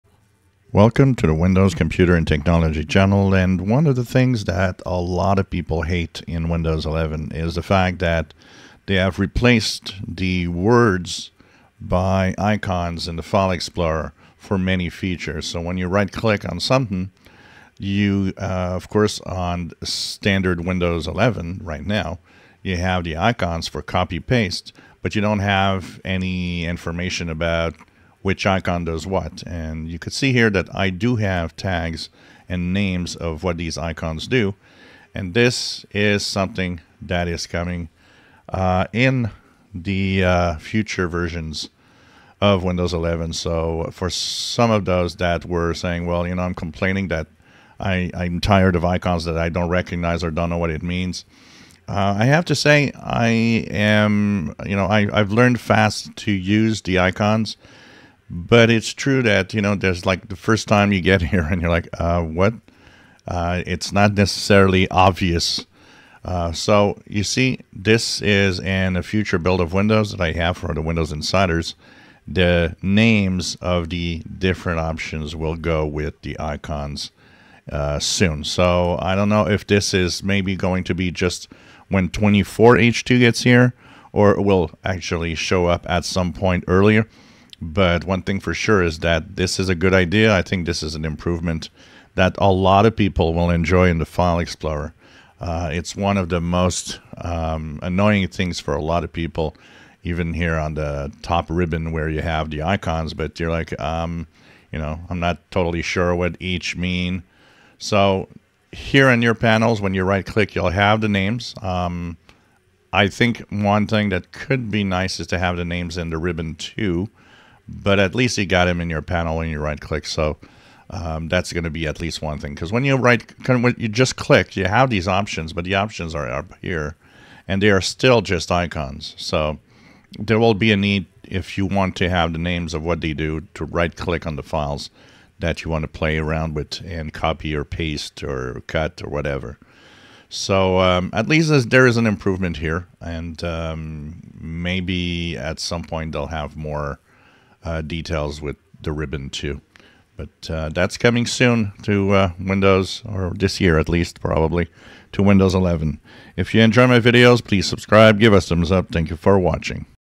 Windows 11 File Explorer improvements coming names under cut copy pase icons menu
This is a much needed improvements for many users of Windows 11